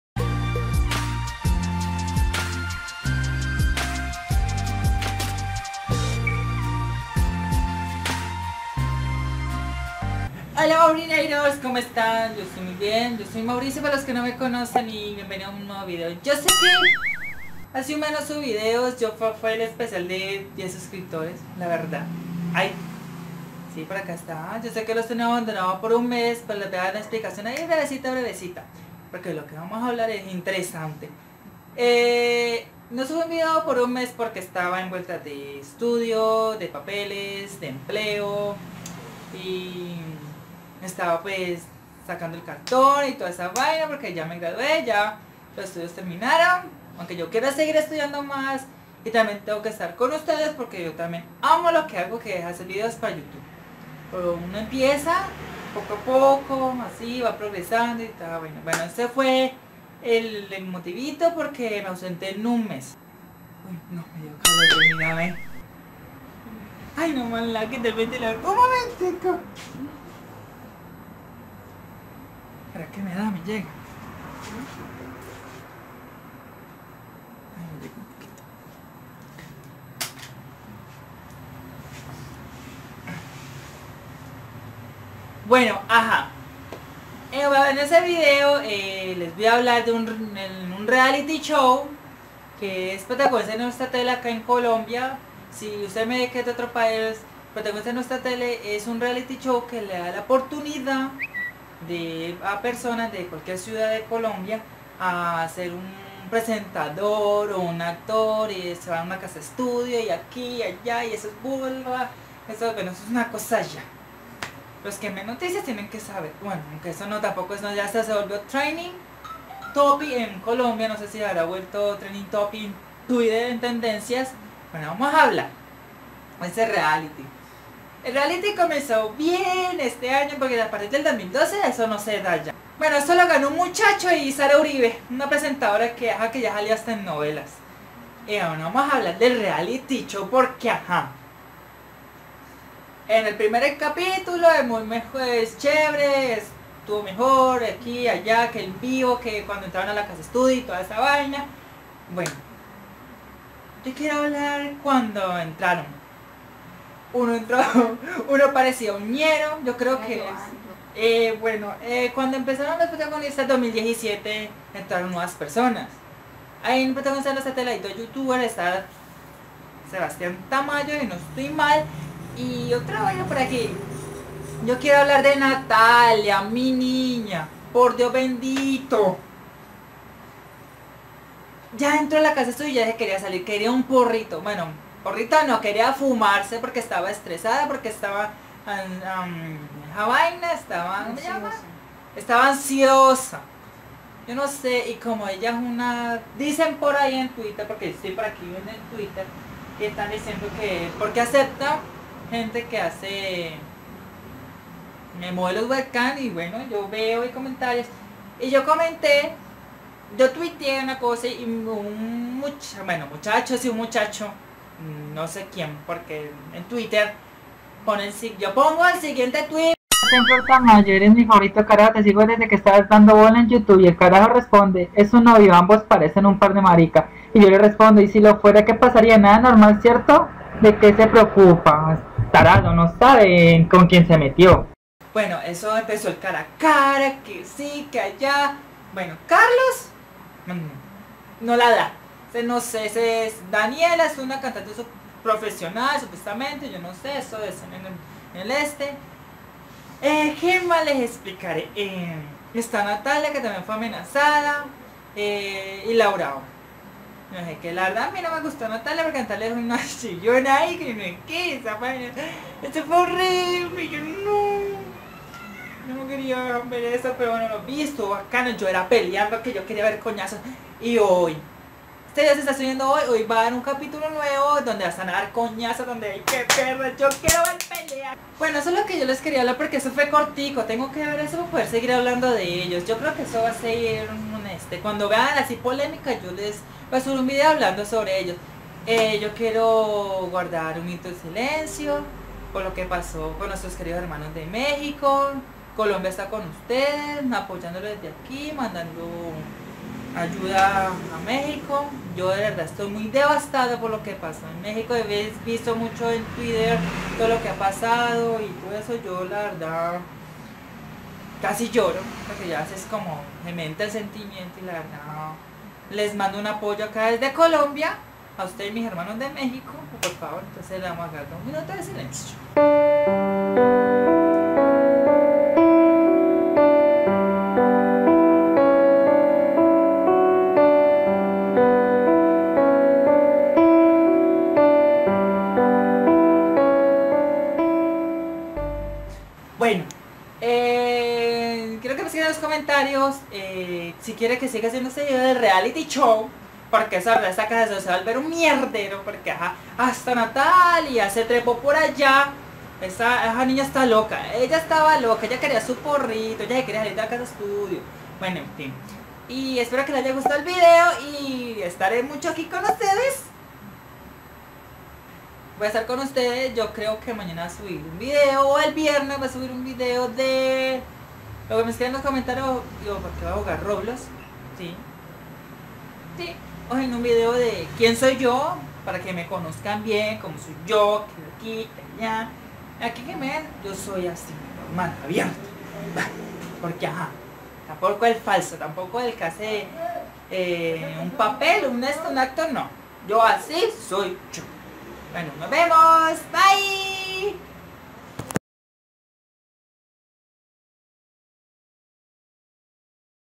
REALITY SHOW (PROTAGONISTAS NUESTRA TELE 2017) | Mauricio Zuñiga
LES HABLARE DE UN REALITY SHOW (PROTAGONISTAS DE NUESTRA TELE 2017) QUE ESO ES UNA GRAN OPORTUNIDAD PARA SER UN ACTOR O ACTRIS ETC. LES EXPLICARE LO QUE VI EN DOS DIAS ESPERO QUE LE GUSTE MUCHO Y LO COMPARTAN CON SUS AMIGOS PARA QUE VAMOS CRECIENDO EN LA FAMILIA DE MAURINEITORS

Muchos Abrazos Aplastantes!
OYE TU, SI TU TE QUIEROOOOOOO!!
Recuerda EL SECRETO DE IR ADELANTE ES COMENZAR Y NUNCA RENDIRSE.